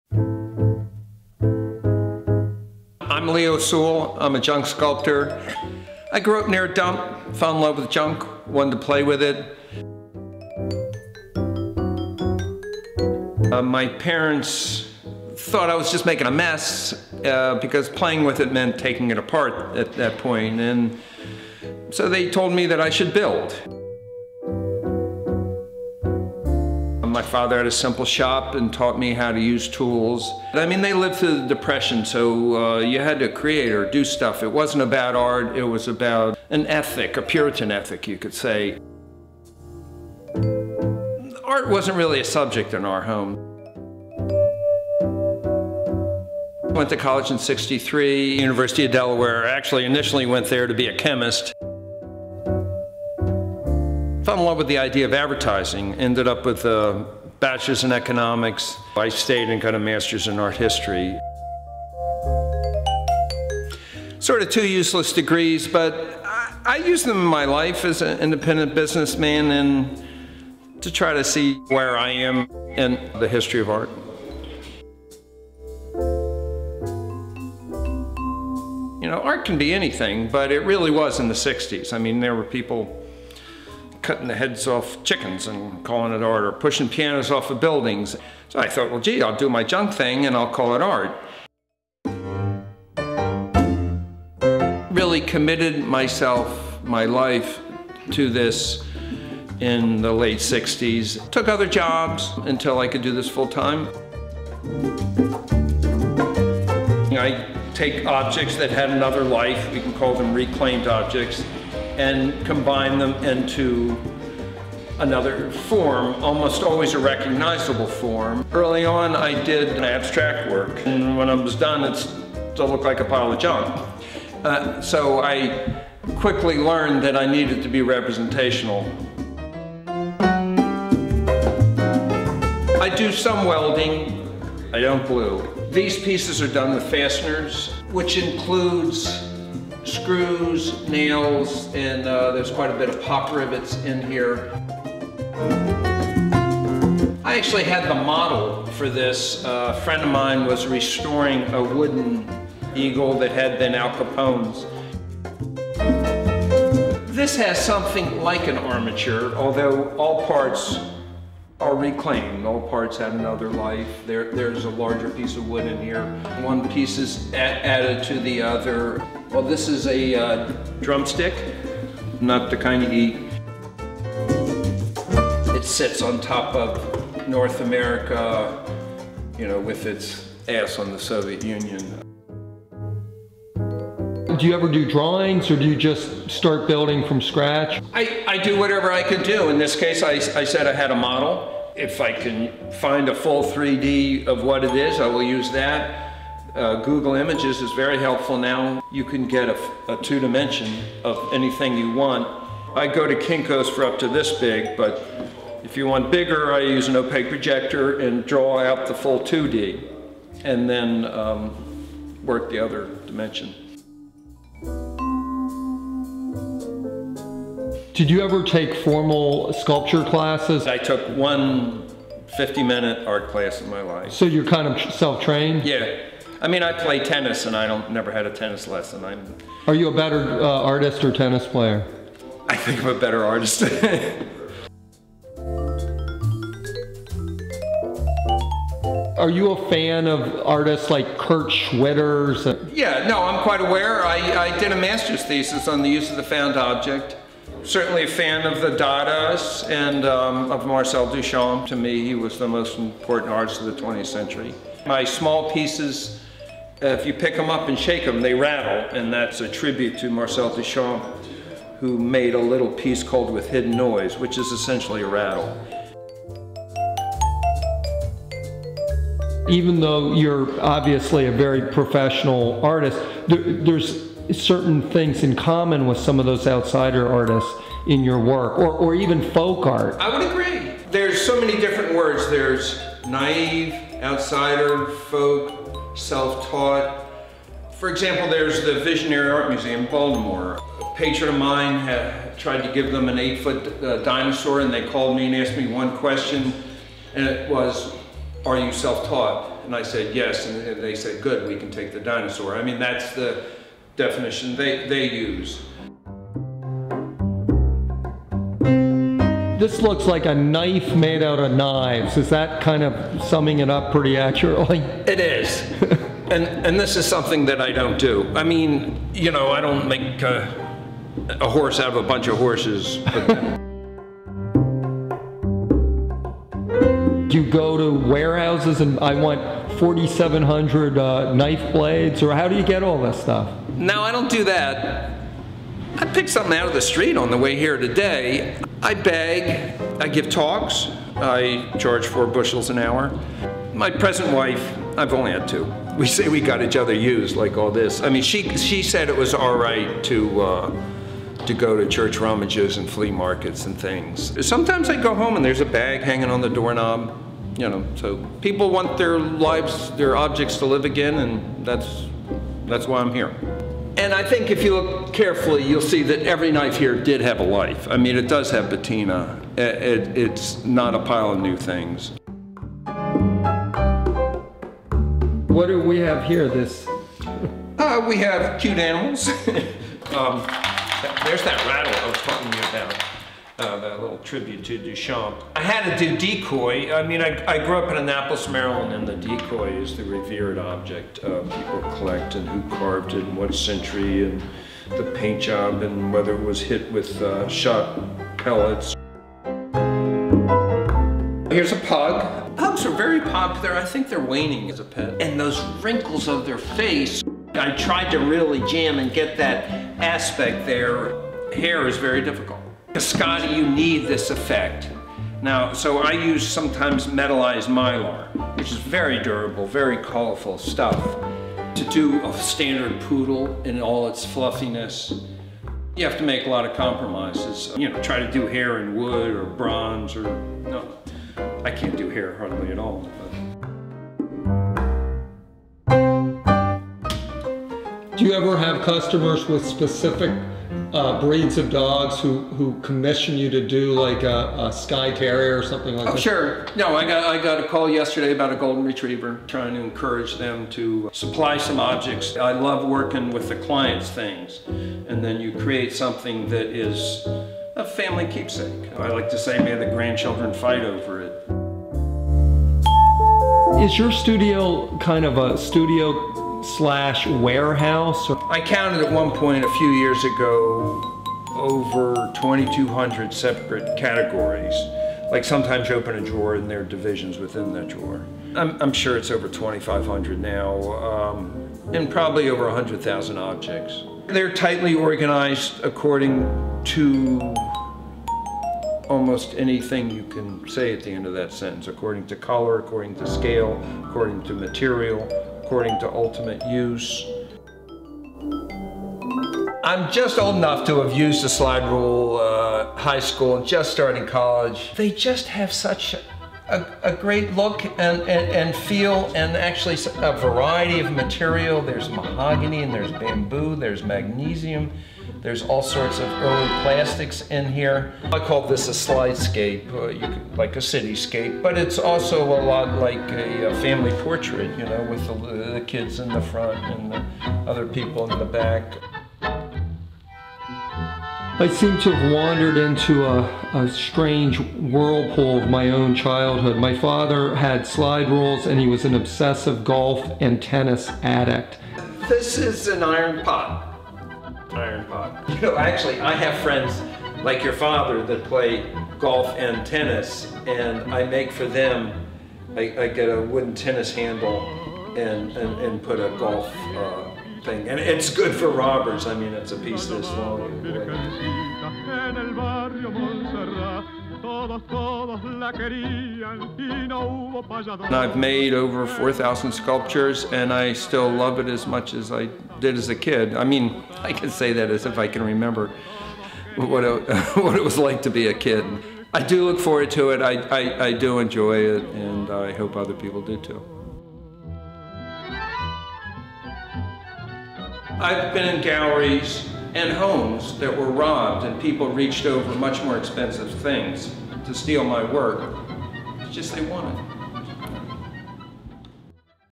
Trash Talkin' with Leo Sewell
Leo Sewell creates naturalistic sculptures out of  a huge variety of found objects. His work and his personality are infused with a gentle humor and his craftsmanship and work ethic are unrivaled. Leo Sewell is a true bridge between the avant garde Dadaist traditions of Marcel Duchamp and the self taught "outsider" visionary artists of America's heartland.